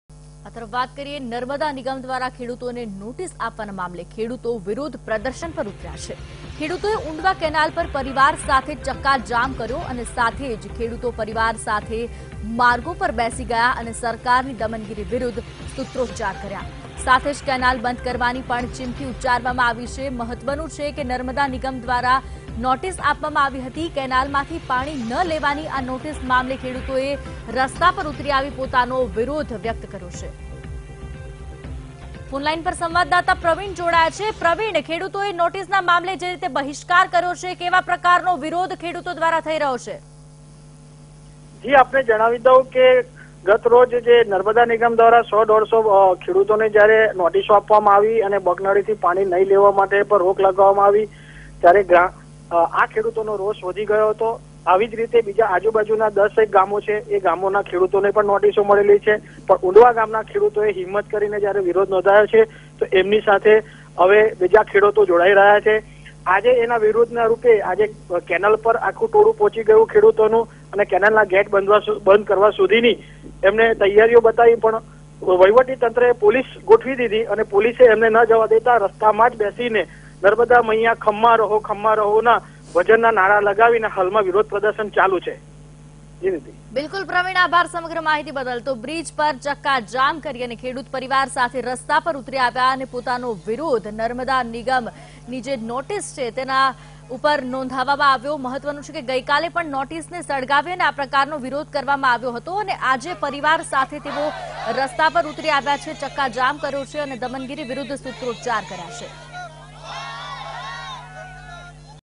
Narmada: Farmers blocked Undva Canal protesting notice by Narmada Nigam | Vtv News
Narmada: Farmers blocked Undva Canal protesting notice by Narmada Nigam